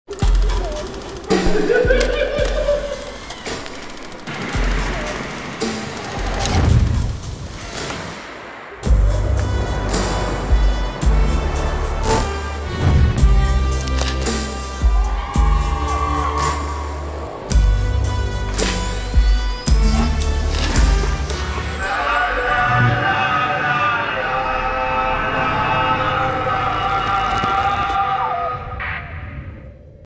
Promospot Filmfestival Oostende 2017
Wim Opbrouck schittert in deze teaser van FFO17 in een regie van Anthony Schatteman!
Filmfestival Oostende 2017 vindt plaats van 8 tot en met 16 september in Oostende!